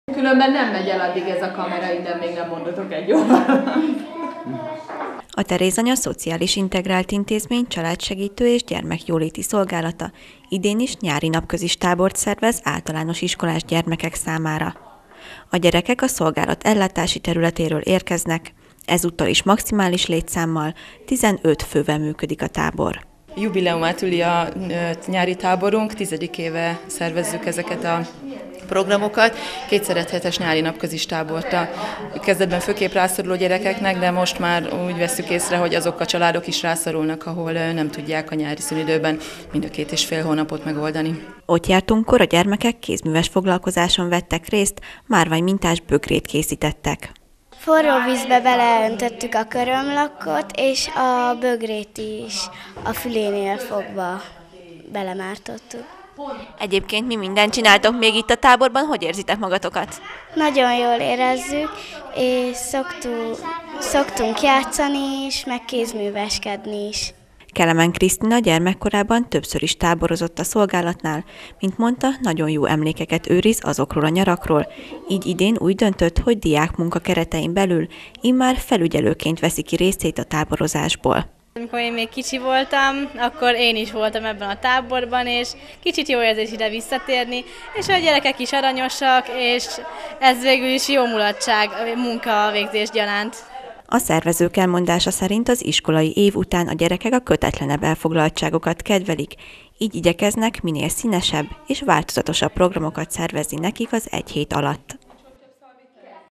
Nyári tábor a Gyermekjóléti Szolgálatnál
Idén is két turnusban fogadják a gyerekeket.